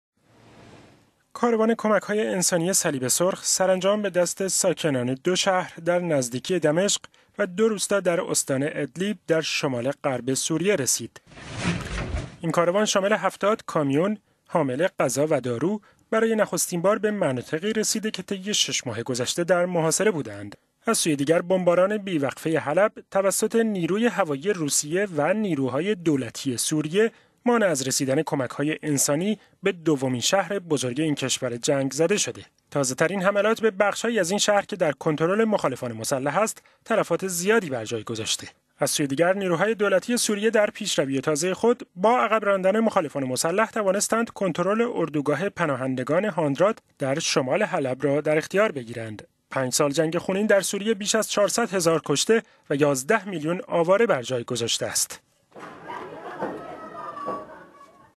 اولین کاروان کمک های انسانی به مناطق تحت محاصره در غرب سوریه رسید
کاروان کمک های انسانی صلیب سرخ سرانجام به دست ساکنان مناطق تحت محاصره در غرب سوریه رسید.

این کاروان شامل هفتاد کامیون برای نخستین بار طی شش ماه گذشته مواد غذایی و دارو را به شهرهای مضایا و زبدانی در نزدیکی دمشق و دو روستای کفرایا و فوا در نزدیکی ادلیب رساندند.

از سوی دیگر بمباران بی وقفه حلب توسط نیروی هوایی روسیه و نیروهای دولتی سوریه مانع از رسیدن کمک های انسانی به دومین شهر این کشور جنگ زده شده است.

تازه ترین حملات به بخش هایی از این شهر که در کنترل مخالفان مسلح است تلفات زیادی برجا گذاشت…